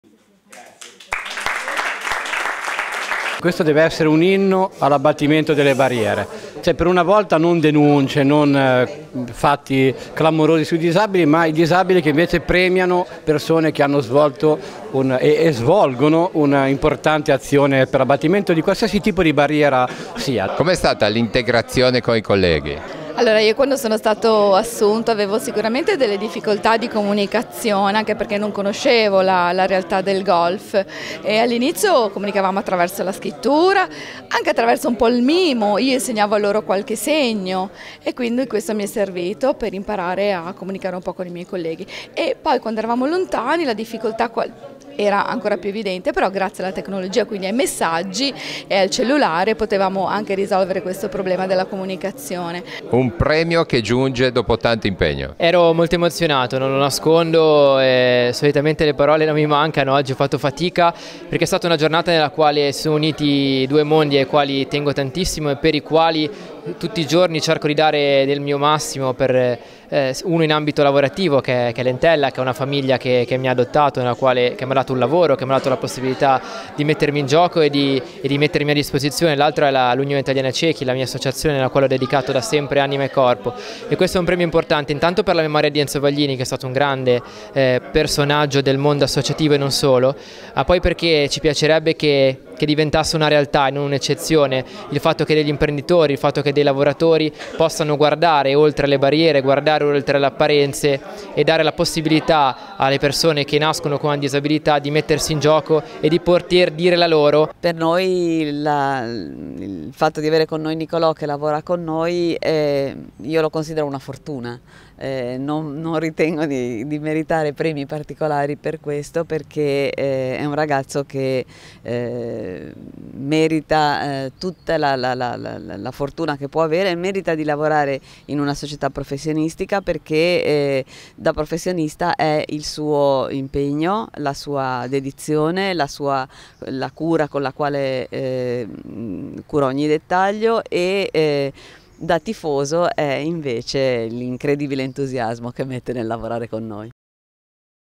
Premio "Una mente sopra le barriere" 2019 ad Antonio Gozzi e Attilio Riola
Sono andati al presidente della Virtus Entella Antonio Gozzi e al presidente del Circolo Golf e Tennis di Rapallo Attilio Riola i premi “Una mente sopra le barriere 2019” dedicato alla memoria del  Dirigente dell’Unione Italiana Ciechi Enzo Vaglini. La cerimonia di consegna della prima edizione del riconoscimento, patrocinato dalla Regione Liguria e ideato dall’Unione Italiana Ciechi, si è svolta a Rapallo.